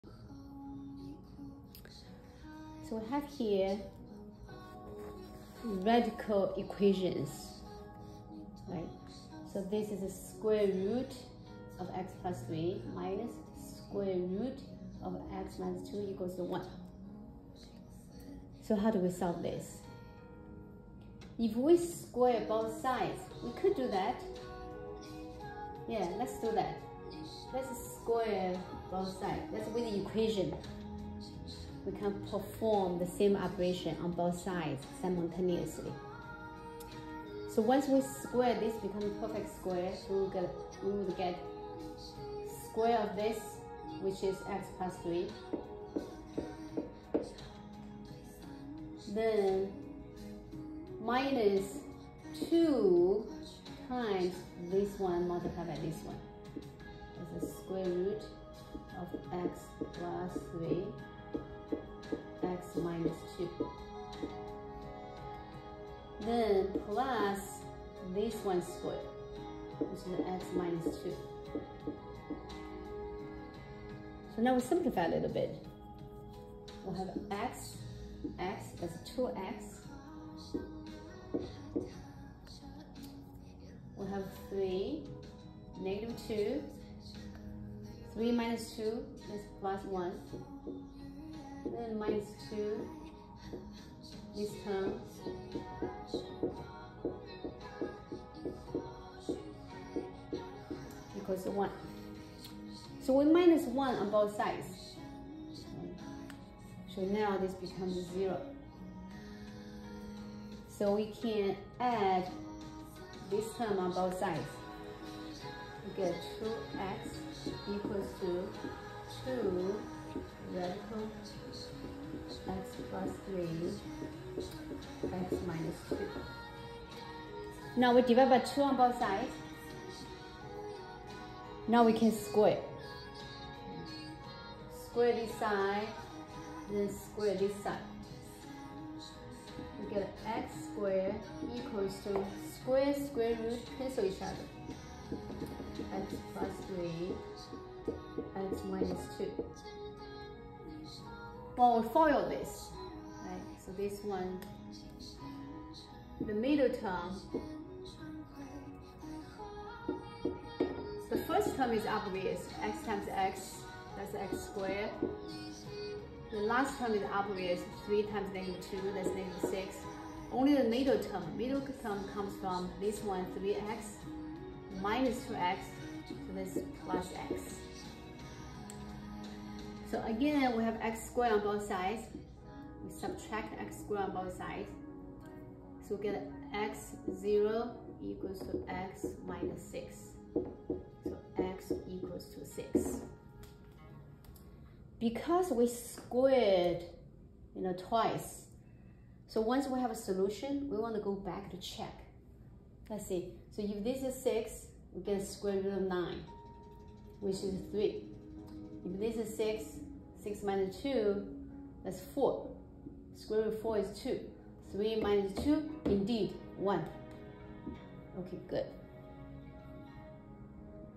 Radical equations#SAT Math
Radical equations' solutions need to be checked, once we square an expression, it creates extraneous solution.